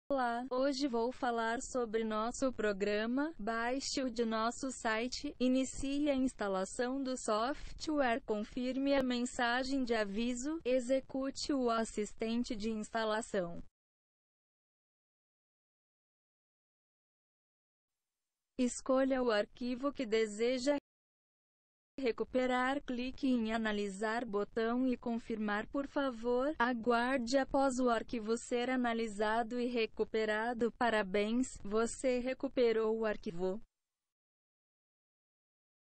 Como reparar arquivo Excel danificado
1. Abra qualquer navegador instalado em seu PC.
2. Na barra de endereço do navegador, anote o endereço e vá para a página do produto.
3. Clique no botão "Download" e aguarde a conclusão do download.
4. Execute o arquivo EXE baixado
5. Instale o programa seguindo as instruções do assistente de instalação.
6. Selecione o arquivo Excel corrompido.
7. Clique no botão "Analisar"
8. Confirme o início do processo de recuperação clicando no botão "Sim". O programa começará a ler e analisar o arquivo por alguns minutos. Após o término da análise, você verá uma tabela com os dados recuperados.
9. Para concluir a recuperação, você deve clicar no botão "Iniciar recuperação". O programa oferecerá salvar os dados recuperados em um novo arquivo do Microsoft Excel ou exportá-lo diretamente para o Microsoft Excel.
10. Aguarde o final do processo de exportação e aproveite os dados recuperados!